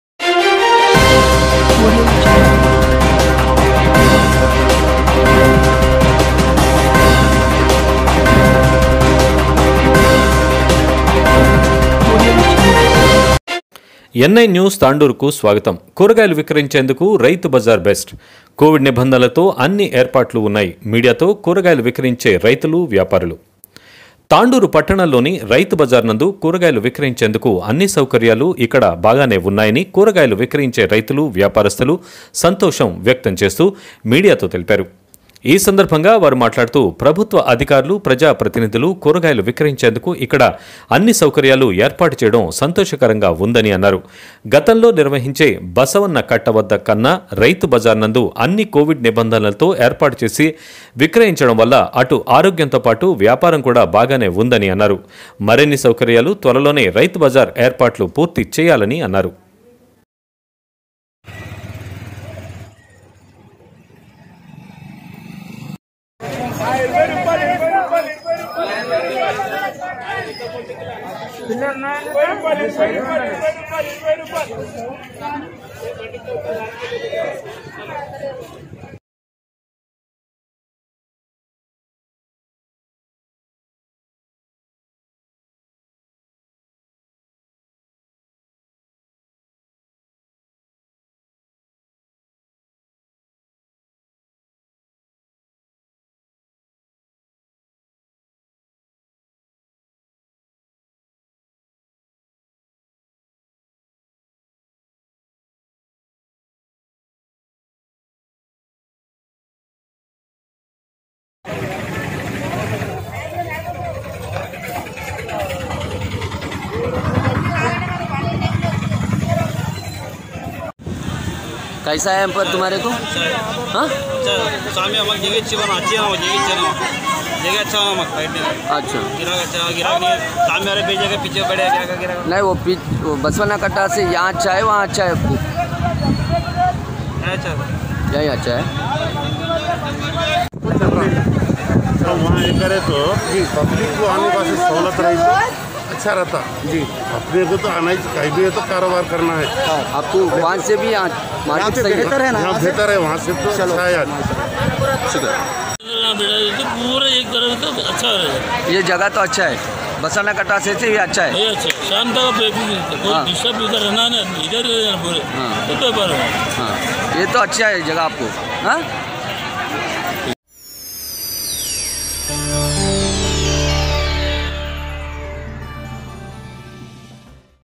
కూరగాయల విక్రయించేందుకు రైతు బజార్ బెస్ట్
N9 న్యూస్ తాండూరు కు స్వాగతం

కోవిడ్ నిబంధనలతో అన్ని ఏర్పాట్లు ఉన్నాయి

మీడియాతో కూరగాయలు విక్రయించే రైతులు, వ్యాపారులు

తాండూరు పట్టణంలోని రైతు బజార్ నందు కూరగాయలు విక్రయించేందుకు అన్ని సౌకర్యాలు ఇక్కడ బాగానే ఉన్నాయని కూరగాయలు విక్రయించే రైతులు వ్యాపారస్తులు సంతోషం వ్యక్తం చేస్తూ మీడియాతో తెలిపారు ఈ సందర్భంగా వారు మాట్లాడుతూ ప్రభుత్వ అధికారులు ప్రజా ప్రతినిధులు కూరగాయలు విక్రయించేందుకు ఇక్కడ అన్ని సౌకర్యాలు ఏర్పాటు చేయడం సంతోషకరం గా ఉందని అన్నారు. గతంలో నిర్వహించే బసన్న కట్ట వద్ద కన్నా  రైతు బజార్ నందు అన్ని కోవిడ్ నిబంధనలతో ఏర్పాటు చేసే విక్రయించడం వల్ల అటు ఆరోగ్యంతో పాటు వ్యాపారం కూడా బాగానే ఉందని అన్నారు. మరిన్ని సౌకర్యాలు త్వరలోనే రైతు బజార్  ఏర్పాట్లు పూర్తి చేయాలని అన్నారు